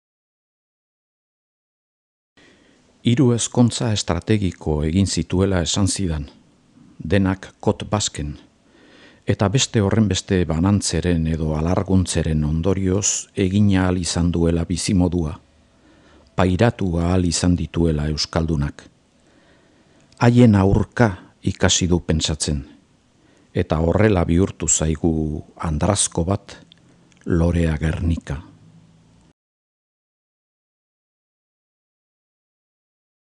Lorea Gernika | book trailer bat.
Lorea Gernika, andrazko bat. Koldo Izagirreren liburu berria.
Trailerraren egileak: Juanba Berasategi eta Imanol Zinkunegi